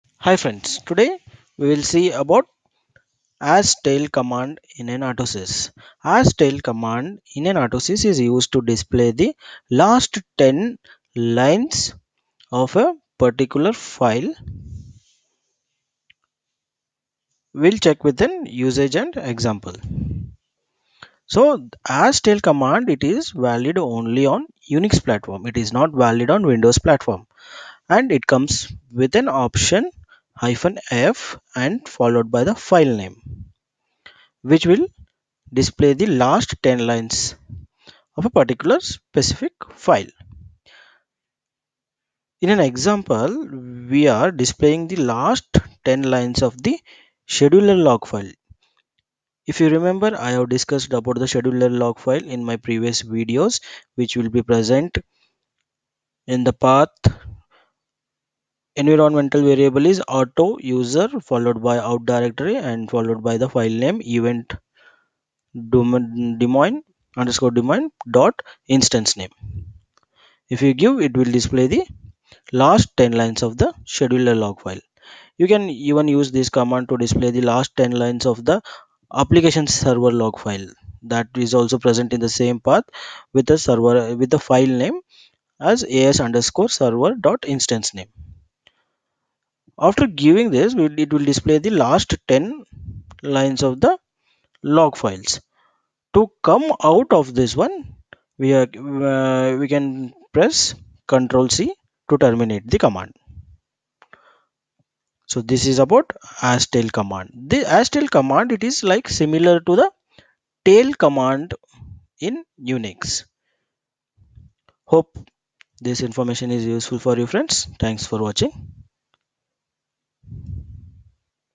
Autosys Tutorial: astail Command—Display End of File
astail Command—Display End of File
Valid on UNIX
The astail command displays the last 10 lines of the file specified in the command.
Syntax
This command has the following format:
astail [-f filename] [-x]
-f filename
(Optional) Defines the name of the file to display.
-x
(Optional) Returns the version number of the astail command to standard output.
Note: To terminate the command, press Ctrl+C.
Example: Display the Last 10 Lines of the Scheduler Log File
This example displays the last 10 lines of the scheduler log file where instance_name is the unique 3-character identifier chosen for this installation.
astail –f  $AUTOUSER/out/event_demon.instance_name